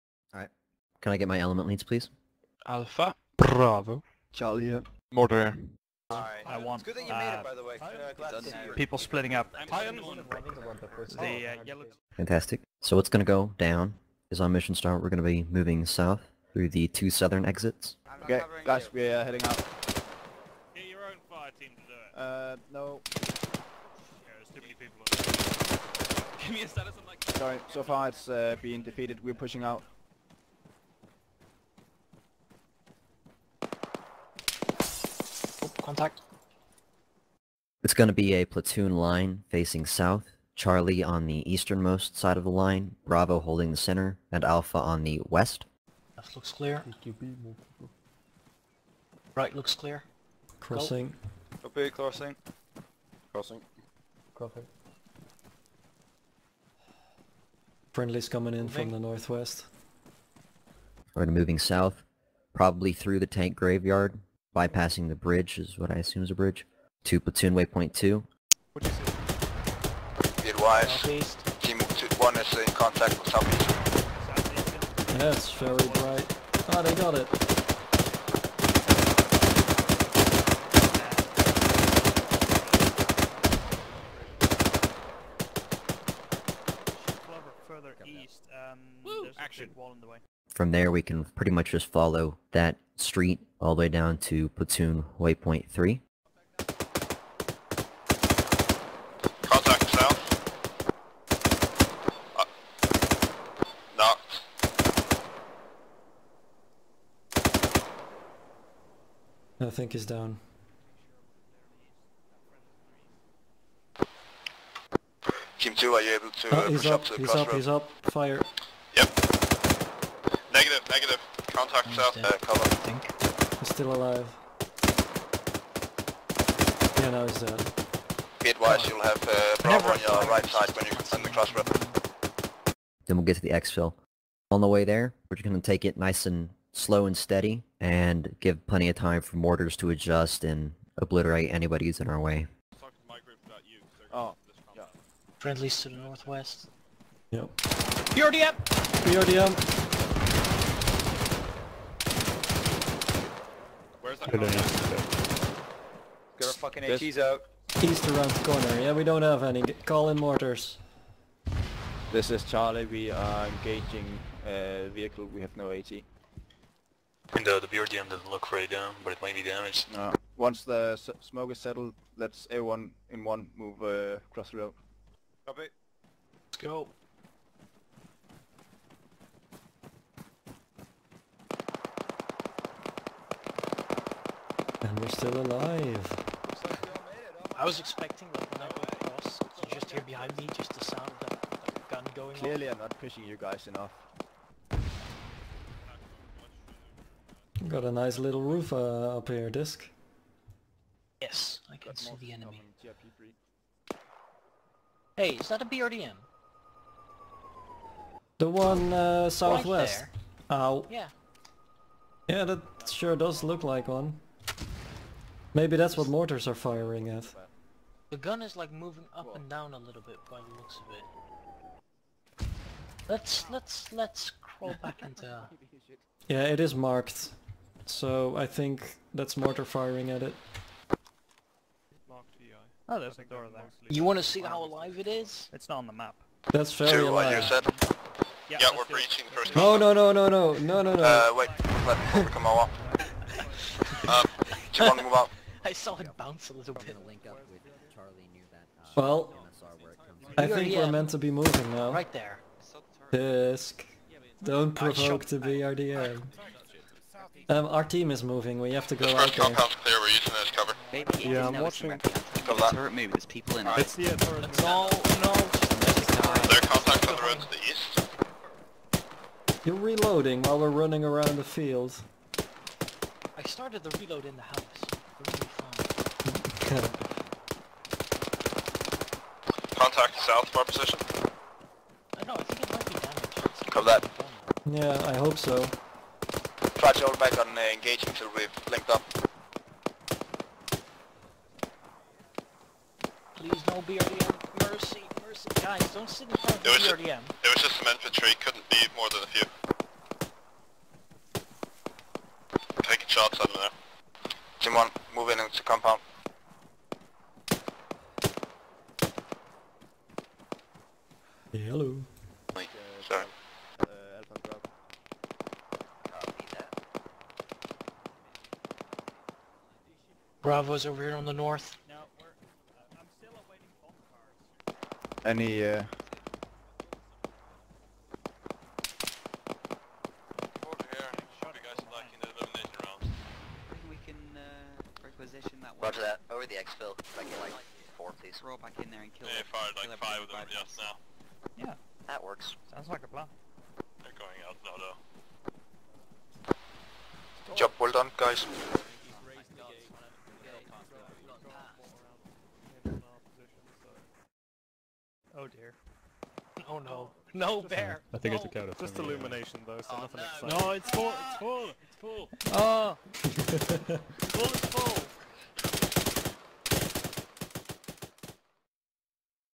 Tea Bag Down: Mortar Mayhem
1Tac public session 01-27, Tea Bag Down on Kidal by BedPleyar.

A summary from my perspective in Charlie section.